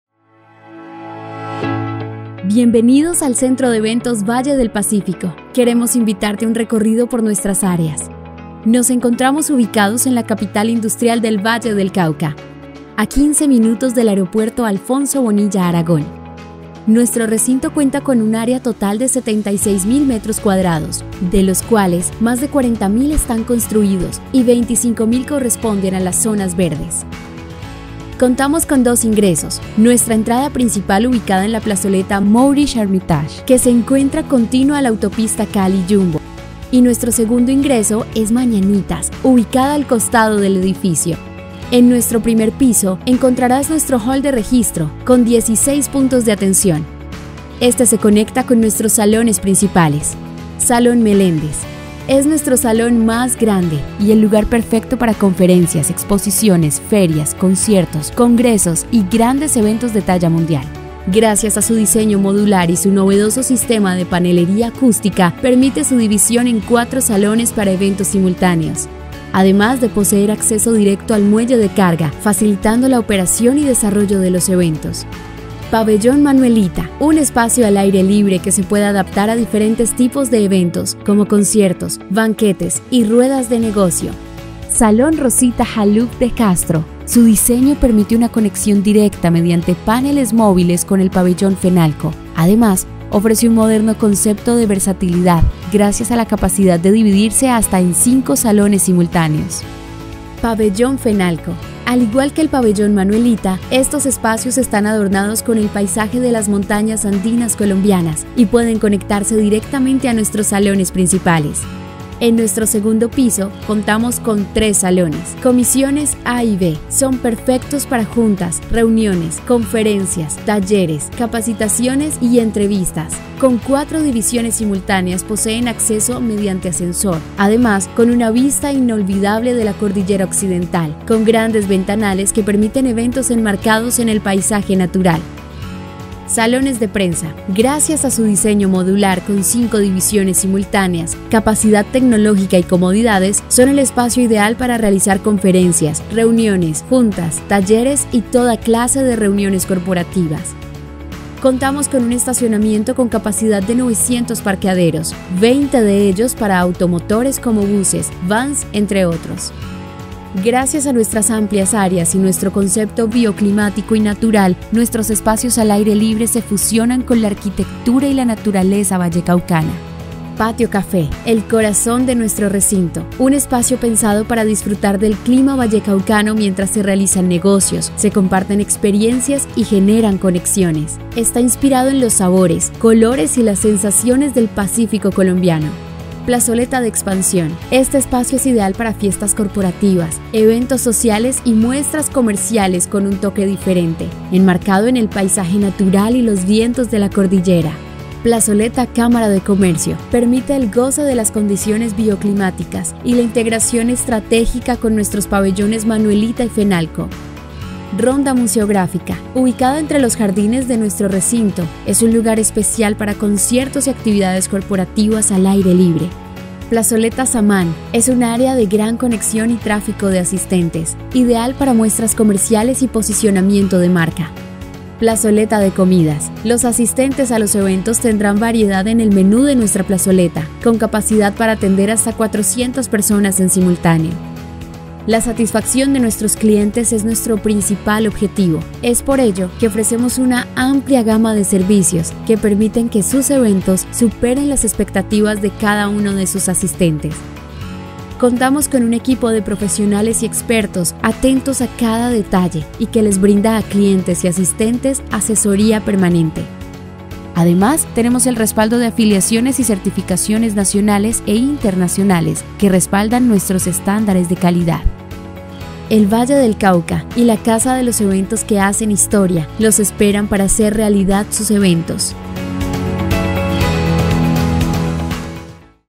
Video Áreas - Centro de Eventos Valle del Pacífico
Bienvenido al Centro de Eventos Valle del Pacífico. Queremos invitarte a conocer los espacios multiformato del recinto para eventos más grande y completo del suroccidente colombiano.